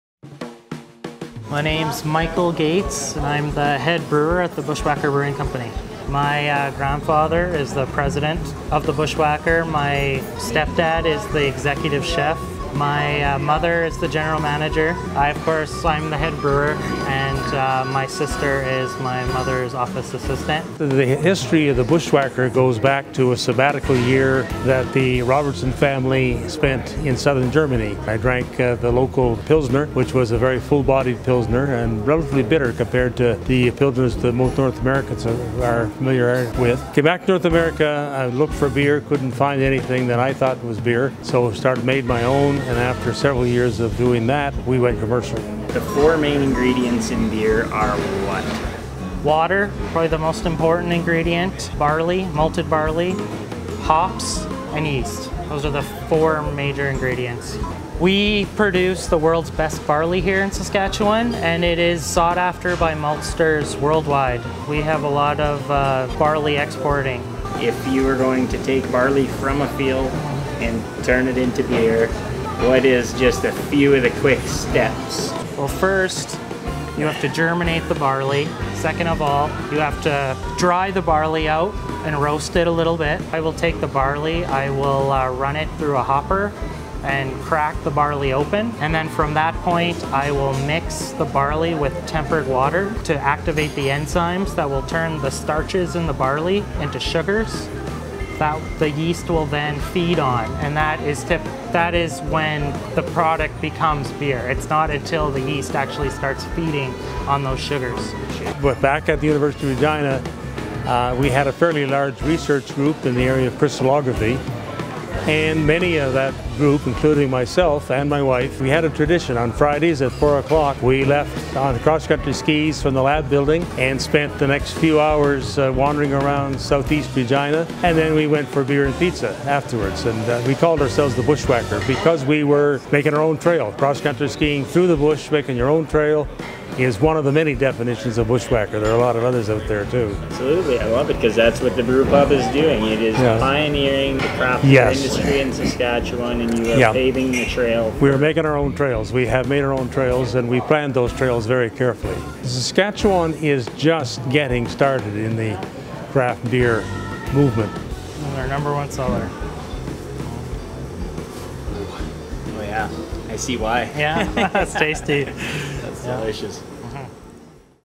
Bushwakker Brewing Company
The Bushwakker opened its doors in January of 1991, being one of the first brewpubs in the province, and has been rated one of the top 5 brewpubs in Canada by the Globe and Mail. With several award winning beers on tap, the family owned and operated brewpub has hundreds of die-hard regulars and continues to draw in customers from all over the world.

Photos courtesy Bushwakker Brewpub